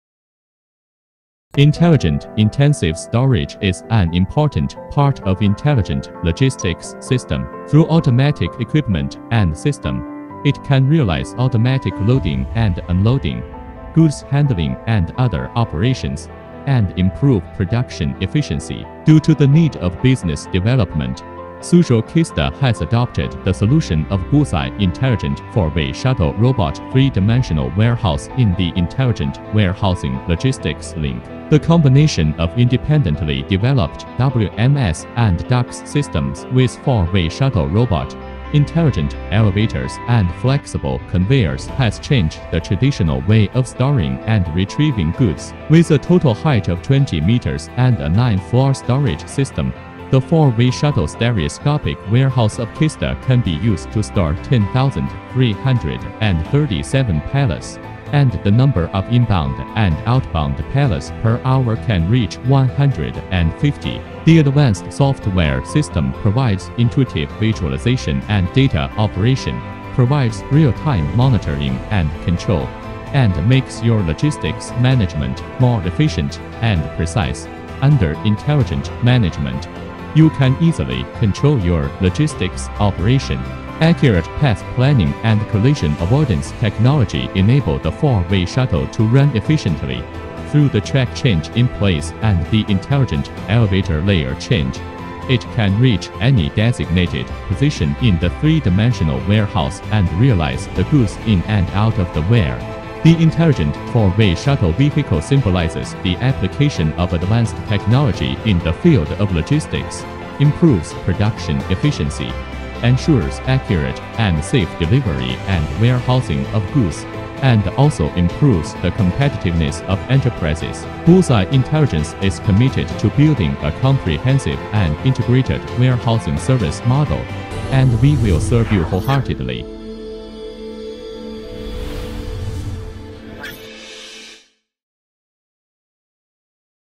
4-Ways Shuttle | Multi-Directional Shuttle Automation System | ATCOSAT
Revolutionizing Transportation: Introducing the 4-Ways Shuttle Automation System! 🚀🌟

Join us on a thrilling journey into the future of transportation with our groundbreaking 4-Ways Shuttle Automation System! 🚗🤖 Discover how this cutting-edge technology is set to transform the way we move, making commuting faster, safer, and more efficient than ever before. 🌐💨